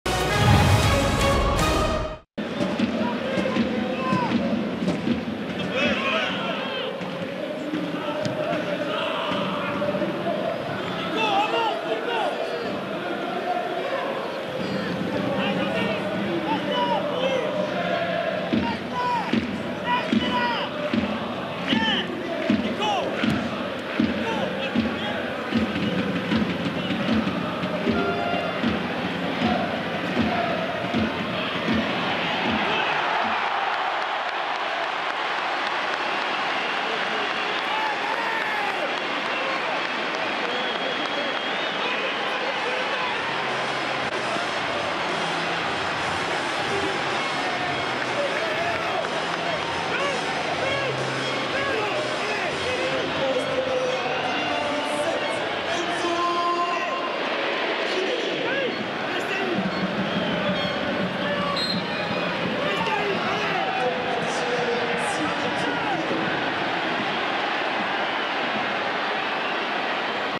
Goal Enzo CRIVELLI (90' +1) / SM Caen - Stade Rennais FC (1-2) (SMC-SRFC) / 2018-19
SM Caen - Stade Rennais FC ( 1-2) Enzo CRIVELLI (90' +1) goal. SM Caen vs Stade Rennais FC Goals in video

Ligue 1 Conforama - Season 2018/2019 - Week 12 Stade Michel d'Ornano - Saturday 3 November 2018

Goals : Adrien HUNOU (60' - Stade Rennais FC) / Ismaila SARR (69' - Stade Rennais FC) / Enzo CRIVELLI (90' +1 - SM Caen)